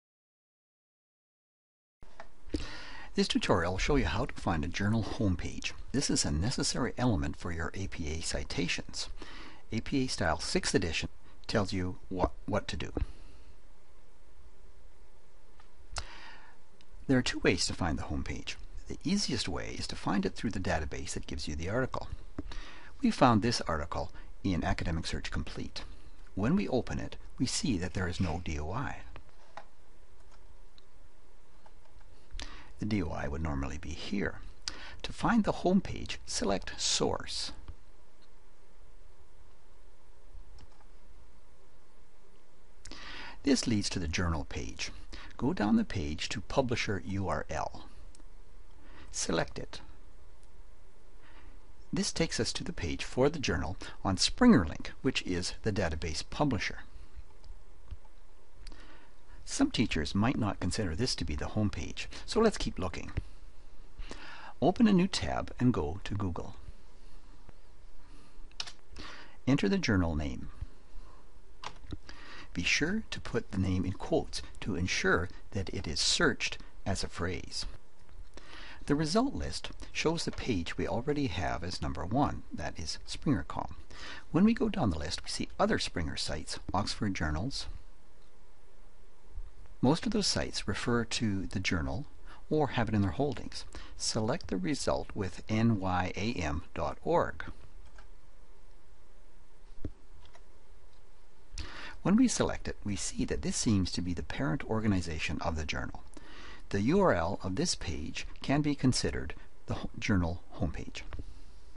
APA - finding a journal homepage
In accordance with the APA Style (6th Edition), articles references from journals that do not use Digital Object Identifiers(D.O.I.) should refer to the journal homepage. To methods of identifying the homepage are demonstrated: using links in the database record or using a search engine (i.e. Google).  Originally created by Richard Baer (Camosun College) on April 21, 2011 using Camtasia Studio. Approximately 1:50 minutes in length. The Camtasia Studio source files and original .mp4 file for this tutorial can be downloaded from the ANTS project repository at the University of Calgary DSpace